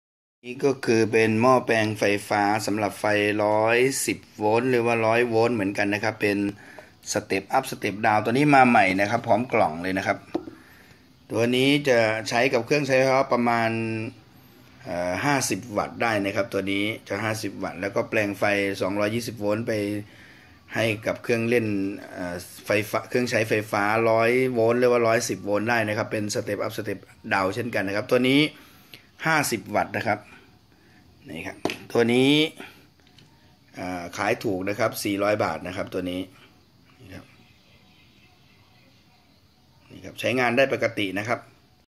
ขายหม้อแปลงไฟ110V. ขายหม้อแปลงไฟฟ้า100V. หม้อแปลงไฟ220เป็น110V. ขายหม้อแปลงไฟ220Vออก100V.
หม้อแปลงไฟ110V สเต็ปอัพ/ดาวน์ แปลงไฟ220V เป็น 100/100V 50W. สำหรับเครื่องใช้ไฟฟ้าจากญี่ปุ่น หรือต่างประเทศ ของใหม่แกะกล่อง รับของเคหะร่มเกล้า หรือนัดพื้นที่ใกล้เคียง สนใจโทร